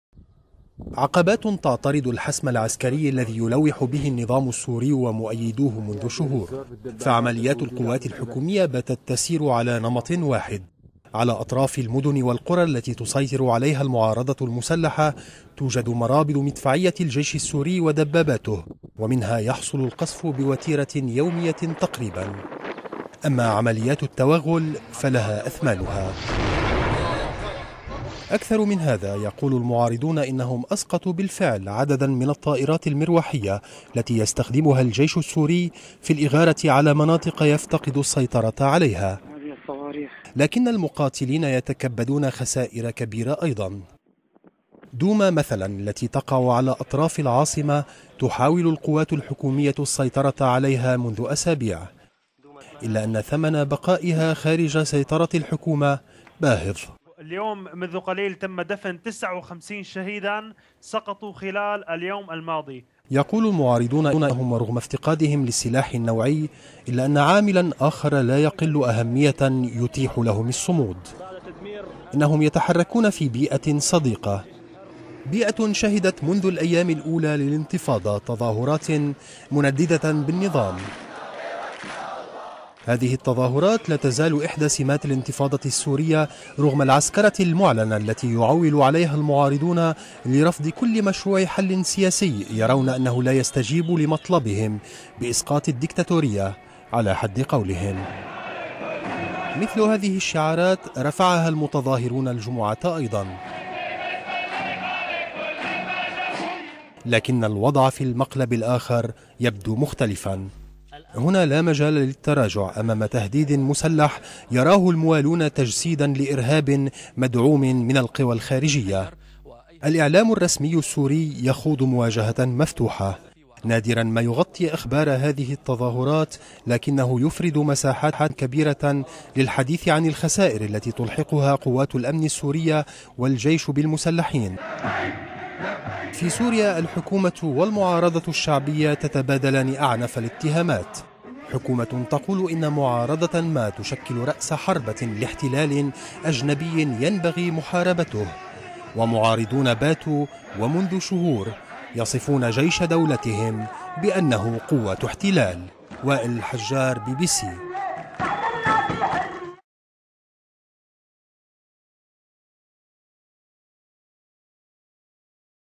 استمرار التظاهرات في انحاء سوريا
قالت لجان التنسيق المحلية السورية إن خمسة وعشرين شخصاً قتلوا اليوم أغلبهم في دير الزور.